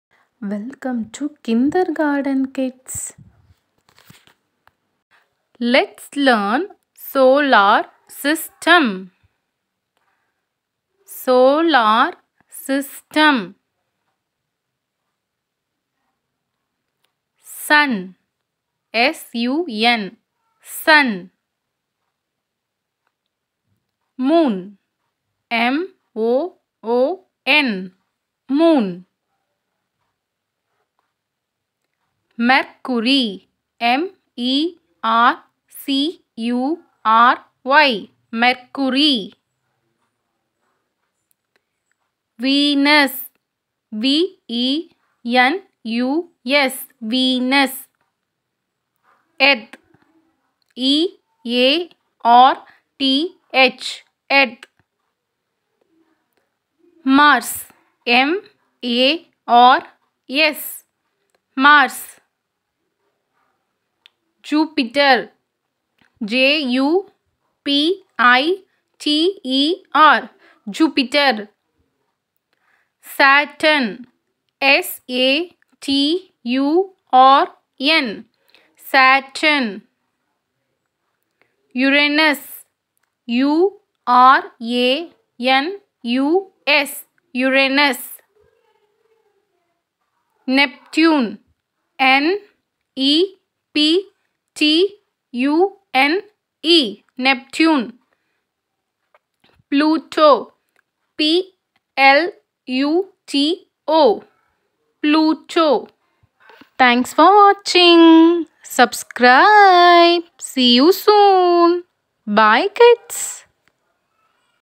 Solar system/ planets name/name of the planet/ galaxy/ sun moon name.
Learn solar system in this video easily through colourful images and background.

This channel moto is to educate kids or children to learn easily through an interesting way like colourful picture, background and letters